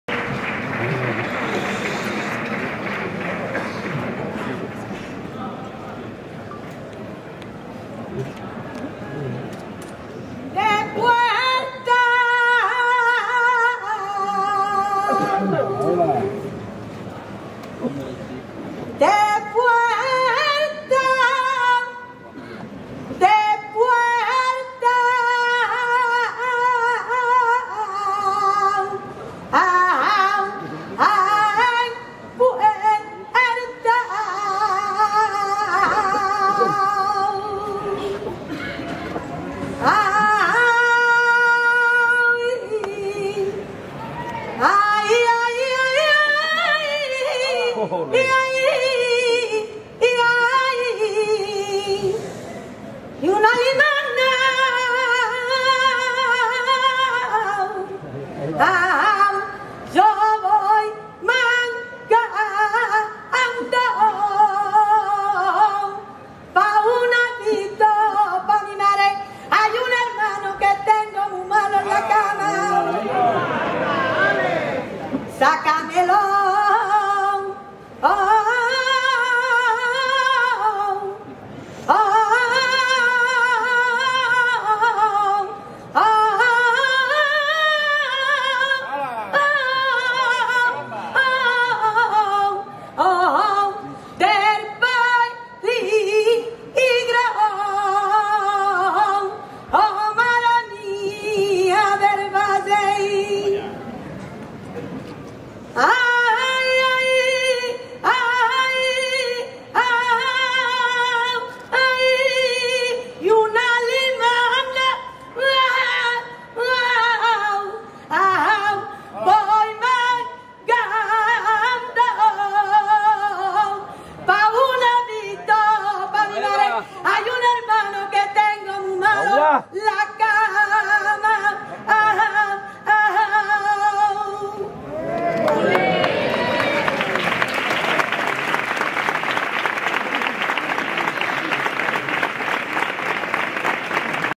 Eva Rubichi canta una saeta a la Virgen del Valle, en Jerez
Momento vivido en la procesión Magna Mariana de 2024 en Jerez de la Frontera